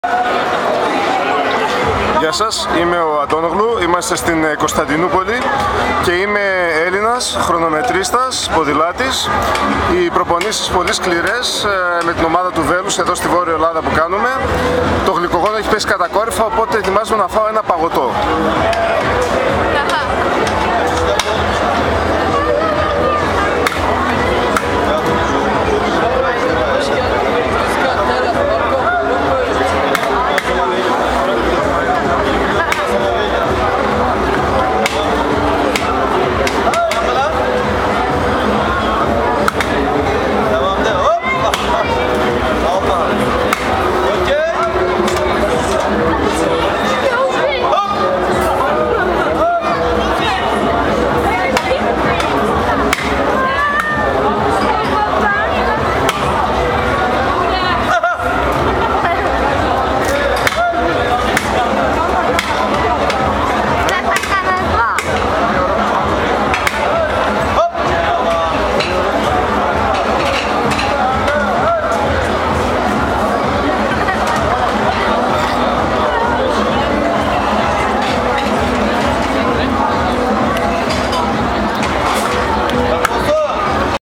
O Antonoglou και το Παγωτό...
Mε λένε Αντώνογλου...

Είμαι ποδηλάτης, Eίμαι Χρονομετρίστας, Είμαι Αθλητής του ΒΕΛΟΥΣ...

Σκληρές προπονήσεις και εξαντλητικές... το γλυκογόνο χαμηλό, θέλω να φάω παγωτό...

Κωνσταντινούπολη το μέρος, πλατεία Taksim... Προσπαθώντας να πάρω το παγωτό μου...